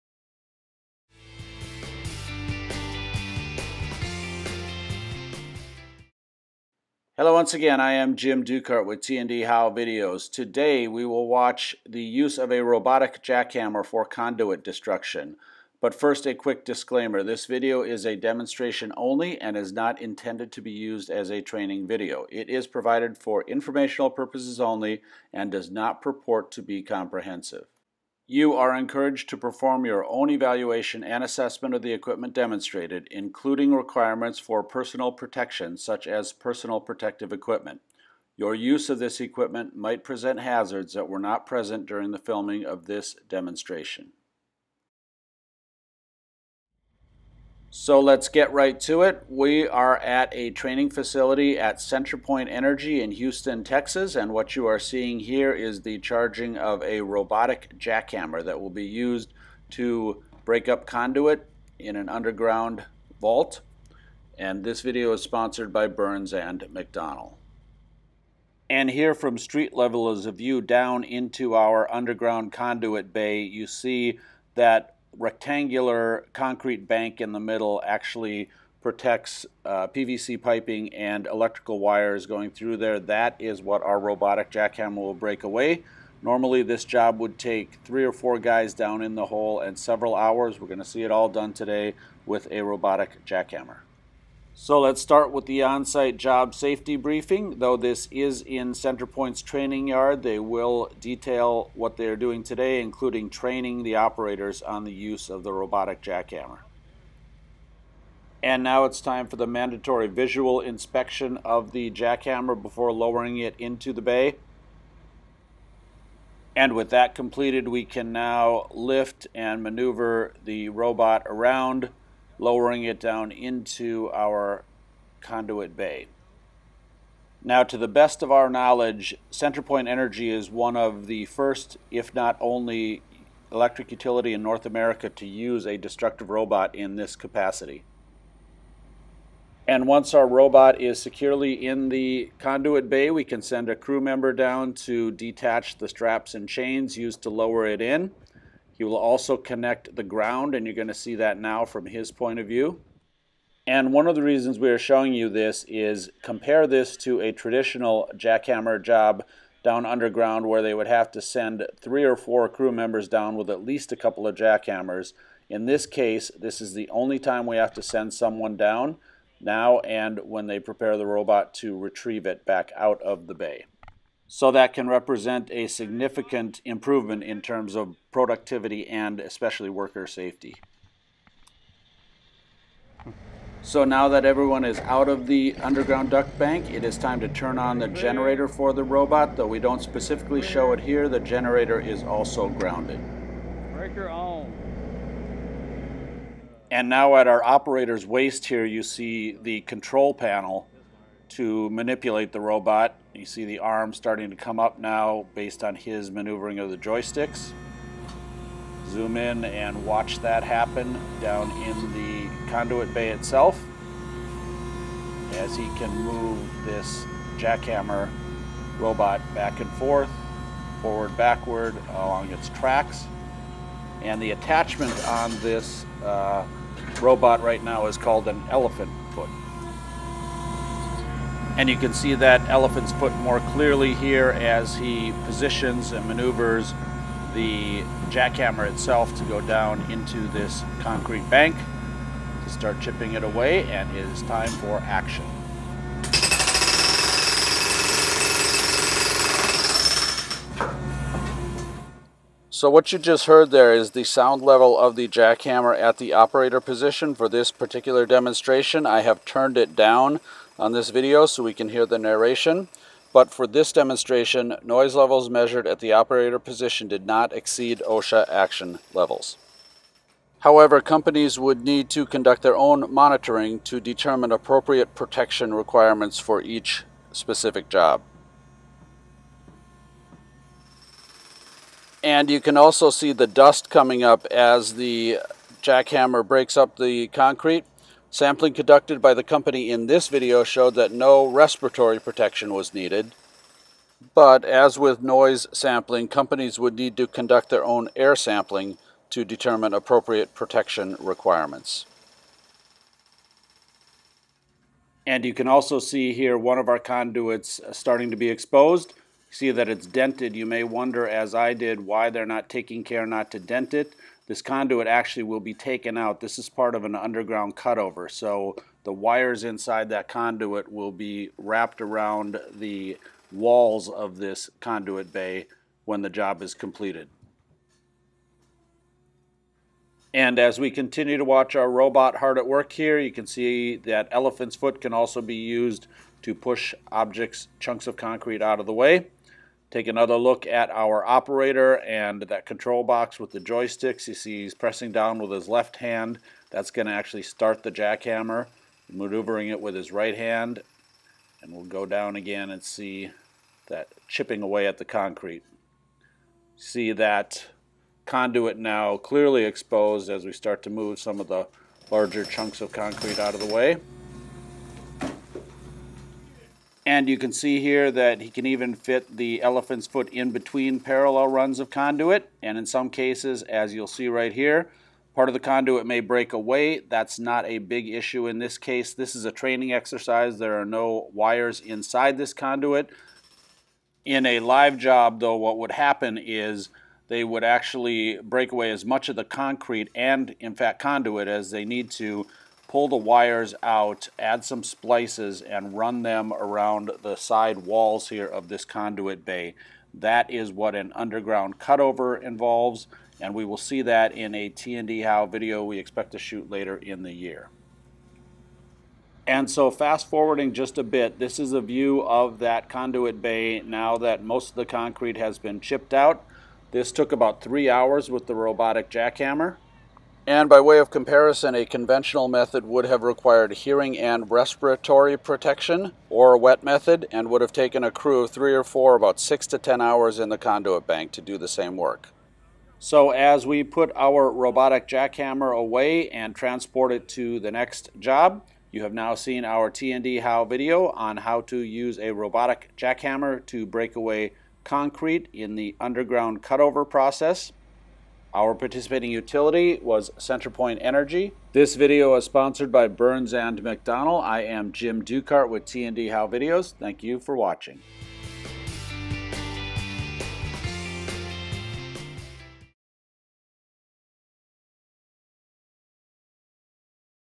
Robotic Jackhammer
Centerpoint demonstrates the use of a destructive robot to break up underground conduit, a necessary first step in an underground cutover. Host utility: Centerpoint Energy